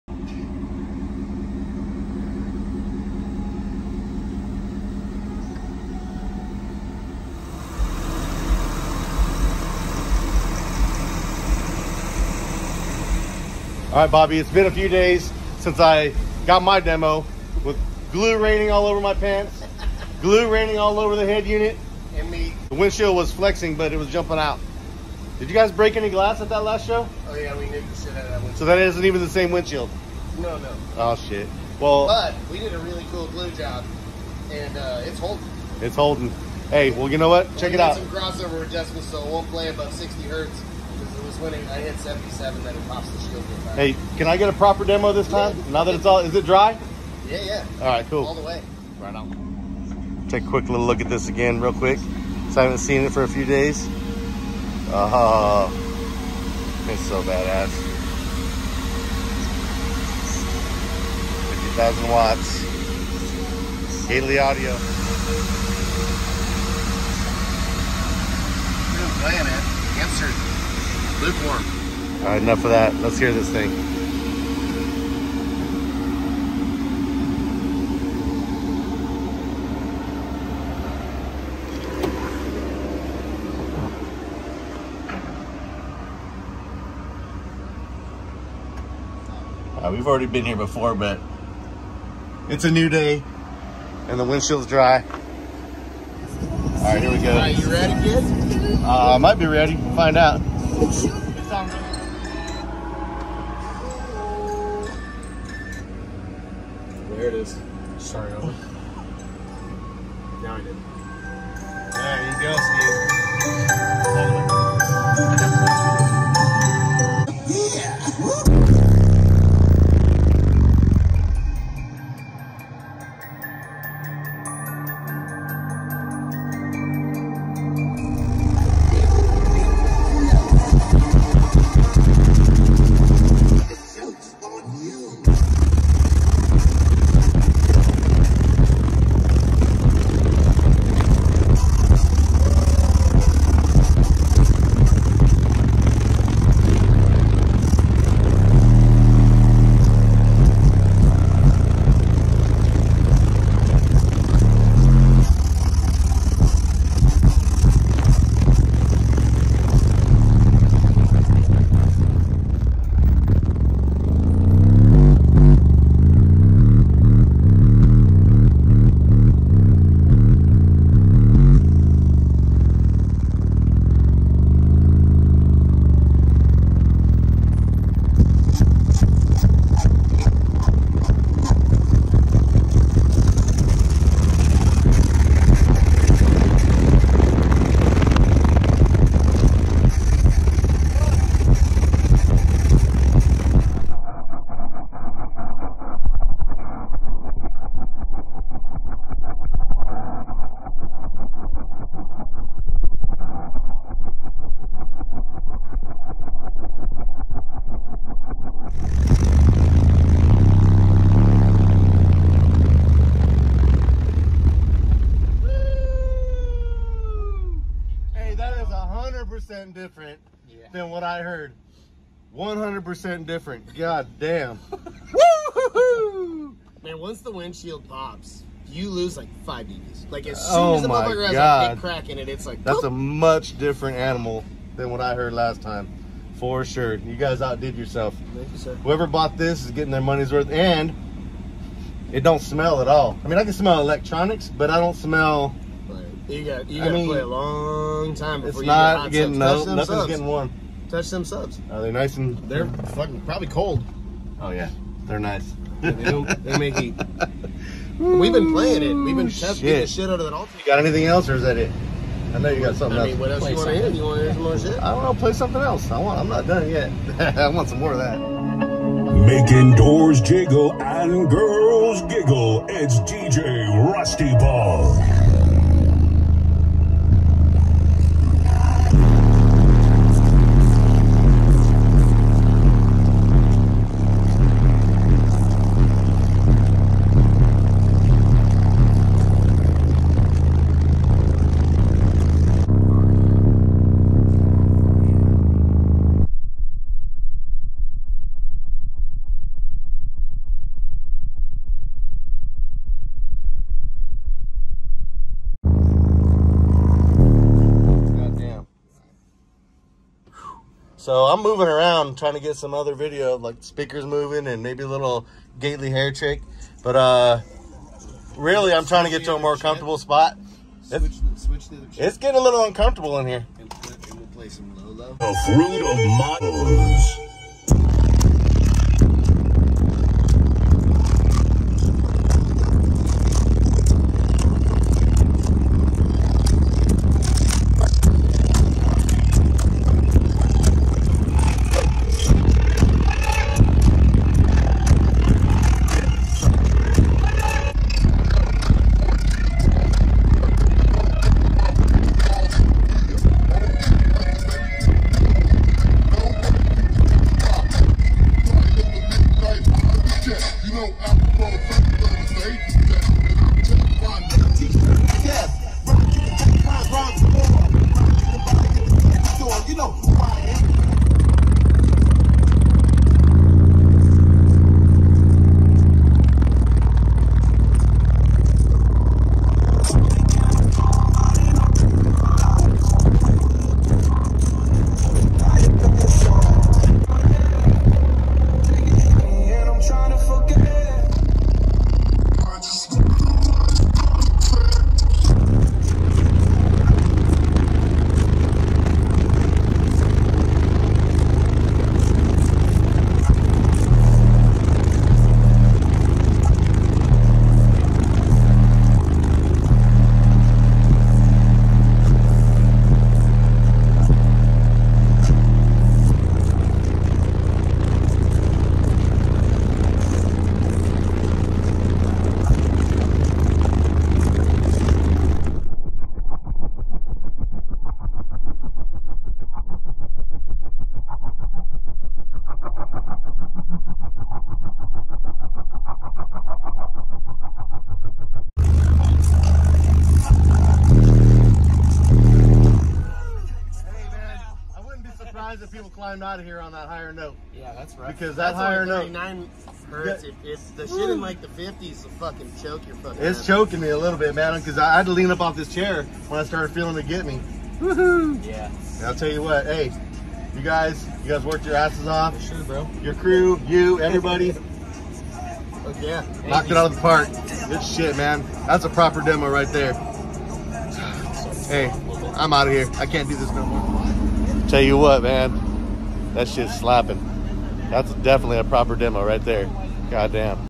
A PROPER BASS Demo 12 15" Subs + 50,000 Watts = Major Ear Flappage! Gately Audio "Soulgazm" Escalade
Now THAT is a proper bass demo!  It sounds 100% different than last time i heard it. Of course that will happen when you lose 5 db while the windshield is venting itself.  What an experience that was, great job Gately Audio and Crew!